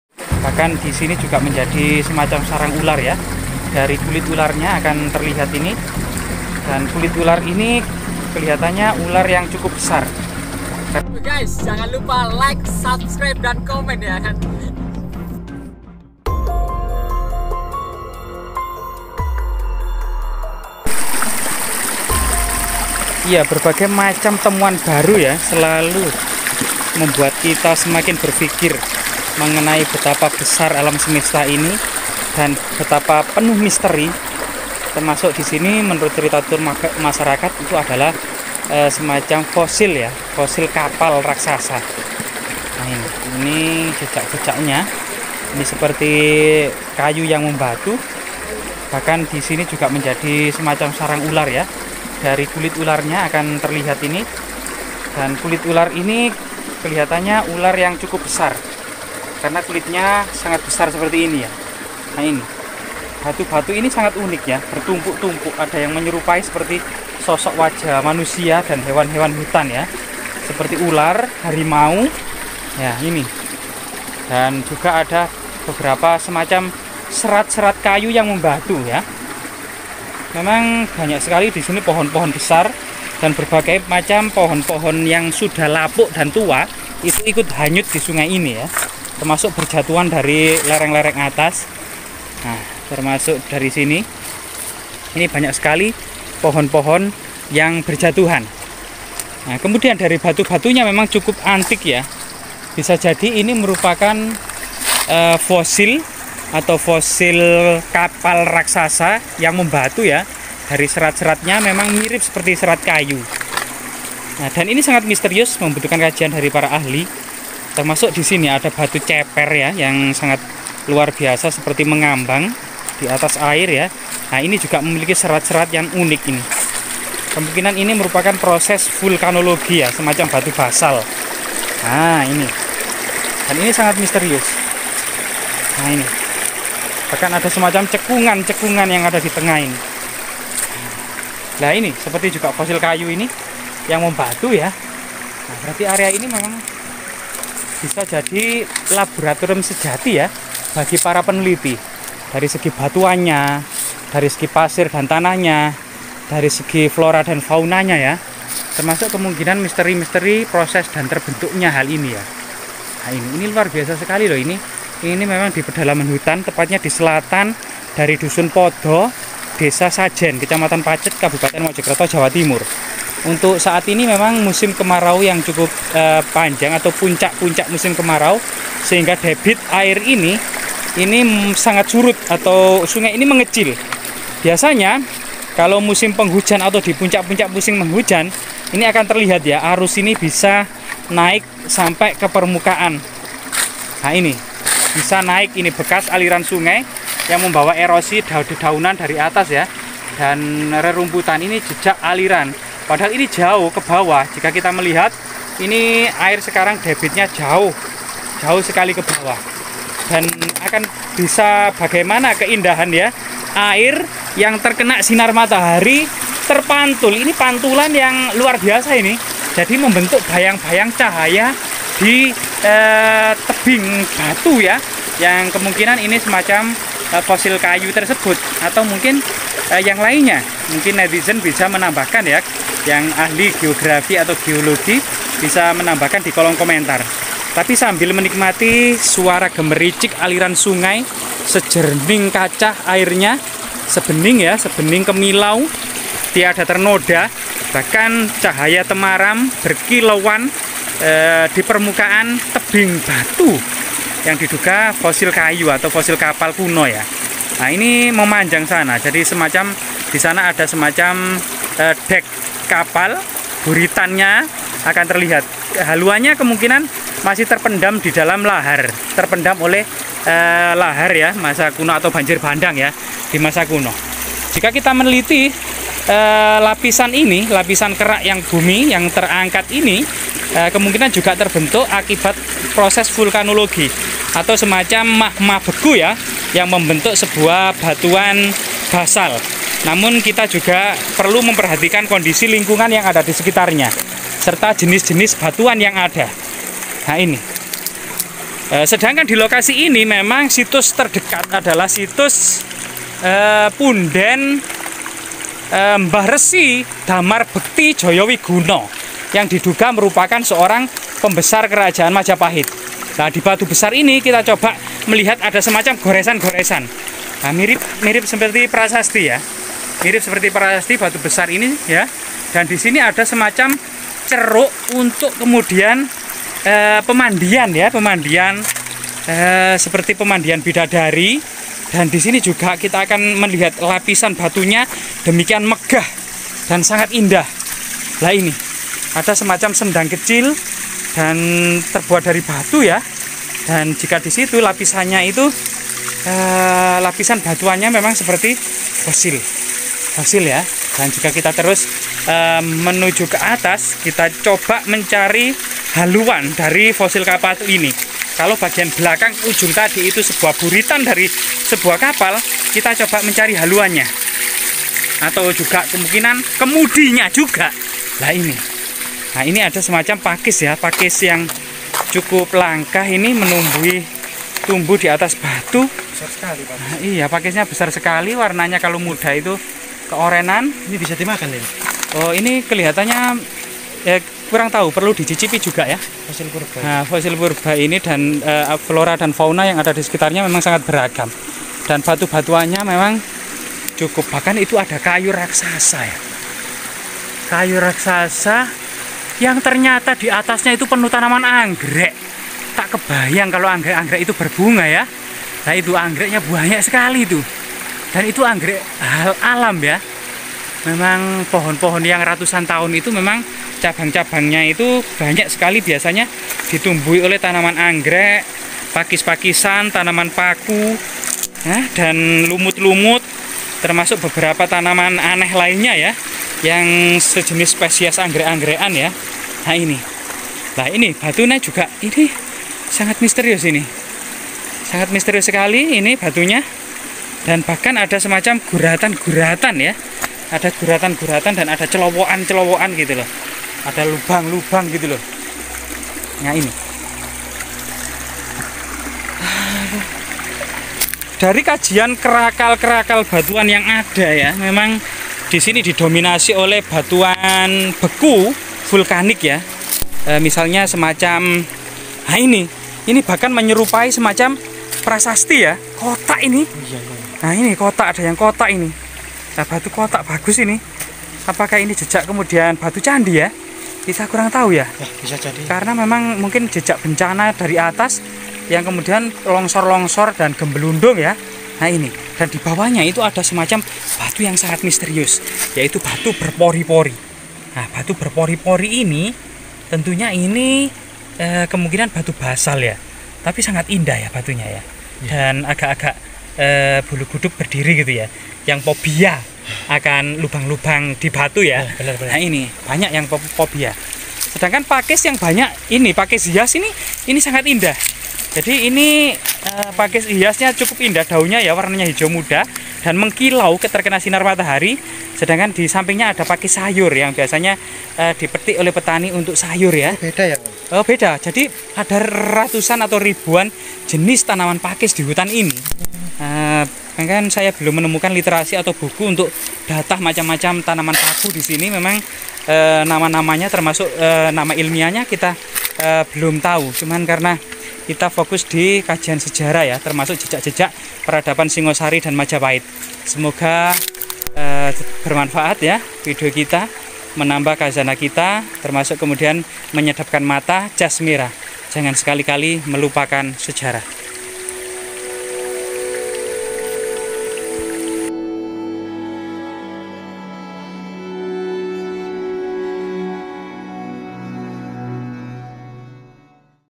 TAK TERDUGA !!! TEMUKAN BENDA MIRIP FOSIL KAPAL NABI NUH MENJADI SARANG ULAR BESAR ?!
Tidak habis-habisnya membahas berbagai fenomena alam dan temuan budaya di atasnya.
Mendapati hal tersebut terkadang kita meneteskan air mata, betapa kecil mungilnya diri ini, dan betapa manja kemayunya kita dibandingkan kehidupan masa lalu.
Berbagai tinggalan masyarakat Nusantara beralam tropis ini tentu didominasi oleh peralatan yang terbuat dari kayu, bambu, rotan, tulang dan kulit binatang, dedaunan, ranting-ranting dan akar-akar pepohonan yang tentu akan lapuk dimakan usia.
Sehingga yang tersisa kebanyakan tinggalan yang terbuat dari batu, logam, tembikar, bata merah dan keramik.
Sedangkan berbagai kejadian di masa lalu terkadang masih menjadi cerita tutur di masyarakat, tertulis di berbagai dokumen historis, di prasasti maupun terdapat dalam kitab suci.
Salah satunya tentang peristiwa banjir besar dan kapal nabi Nuh terdapat dalam semua kitab samawi serta terdapat dalam tulisan Gilgames.
Beberapa negara mempercayai kapal nabi Nuh ditemukan di negaranya misalnya di Turki, Rusia, bahkan di Irak.
Sebuah formasi batu alam menyerupai fosil kapal nabi Nuh juga terlihat di pinggir sungai di desa Sajen, Pacet, Mojokerto, Jawa Timur.
Bahkan kita mendapati banyak kulit ular berteparan di atasnya. Diduga menjadi sarang ular besar
Bagaimana menurut netizen setelah menyaksikan sendiri video selengkapnya di YouTube : Majapahit Study Club ???